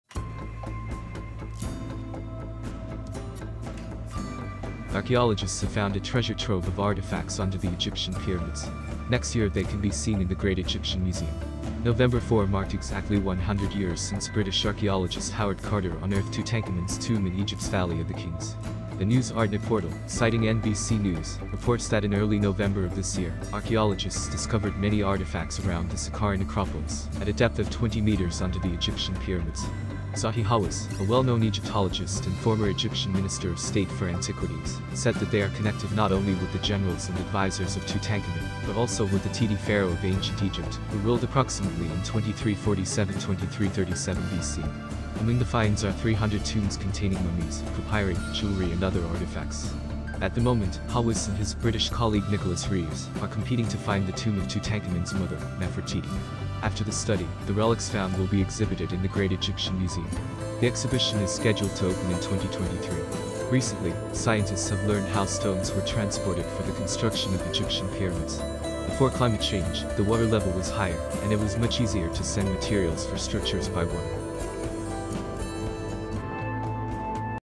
Archaeologists have found a treasure trove of artifacts under the Egyptian pyramids
Next year they can be seen in the Great Egyptian Museum.

November 4 marked exactly 100 years since British archaeologist Howard Carter unearthed Tutankhamun's tomb in Egypt's Valley of the Kings. The News Artnet portal, citing NBC News, reports that in early November of this year, archaeologists discovered many artifacts around the Saqqara necropolis at a depth of 20 meters under the Egyptian pyramids.
Zahi Hawass, a well known Egyptologist and former Egyptian Minister of State for Antiquities, said that they are connected not only with the generals and advisers of Tutankhamun, but also with the Teti - pharaoh of Ancient Egypt, who ruled approximately in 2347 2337 BC. Among the finds are 300 tombs containing mummies, papyri, jewelry and other artifacts.

At the moment, Hawass and his British colleague Nicholas Reeves are competing to find the tomb of Tutankhamun's mother, Nefertiti. After the study, the relics found will be exhibited in the Great Egyptian Museum. The exhibition is scheduled to open in 2023.

Recently, scientists have learned how stones were transported for the construction of Egyptian pyramids. Before climate change, the water level was higher, and it was much easier to send materials for structures by water.